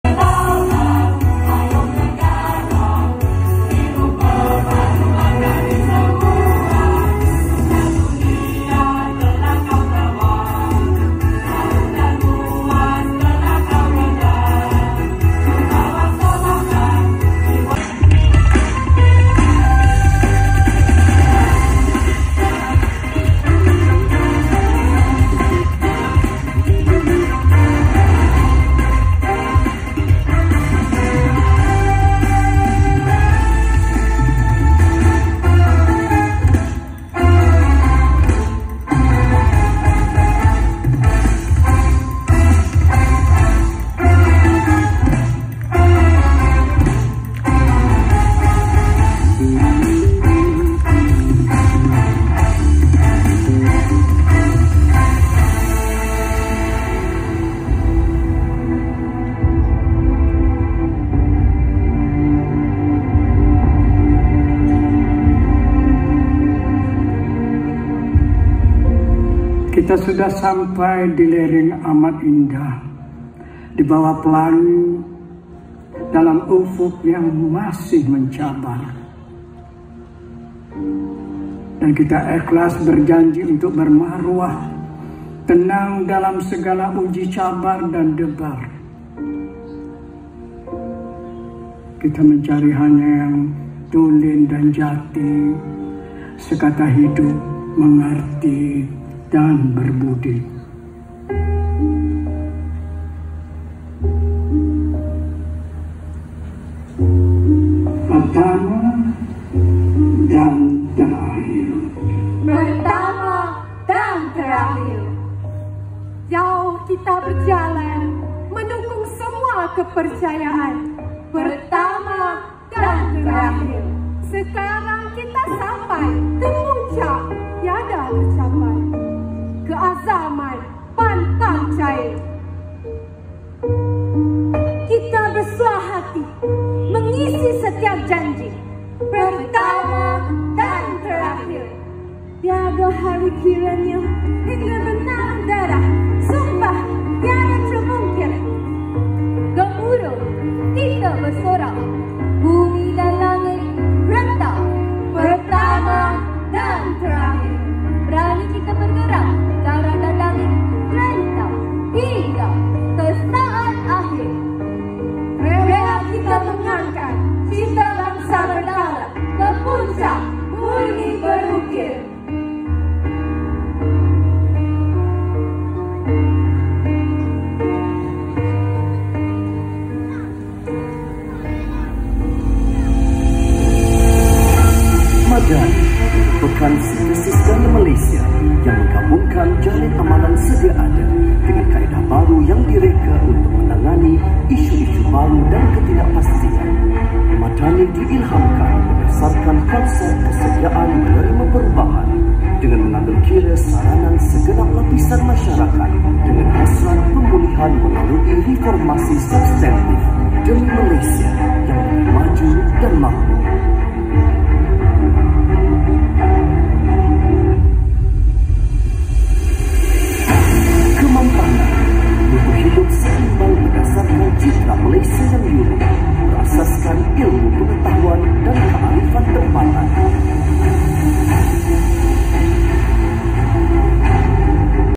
Persembahan Muzikal (live in IPOH 2023)
sesi Raptai PENUH Sambutan Hari Kebangsaan 2023 Peringkat Negeri Perak